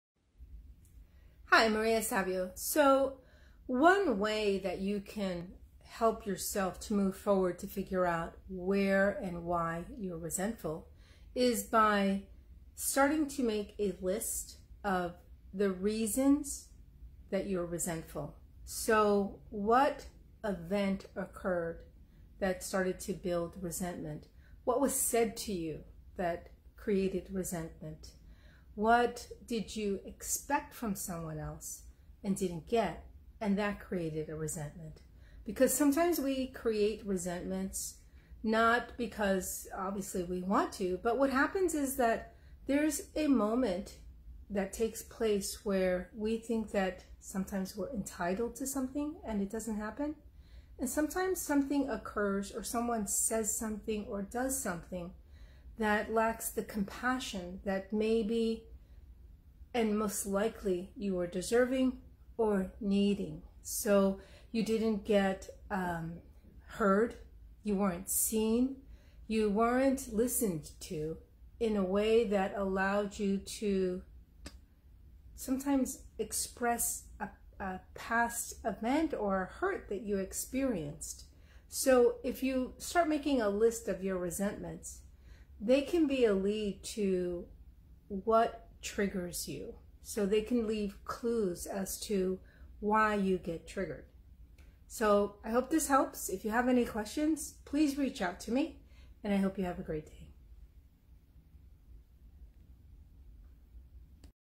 Can your resentments give you clues to your anger
The resentments you have been holding on to can provide you with clues as to why you have anger issues.

SOLUTIONS WITHOUT YEARS OF THERAPY
It doesn't have to take years to change your life or end a bad habit. Lean into change and your life will change.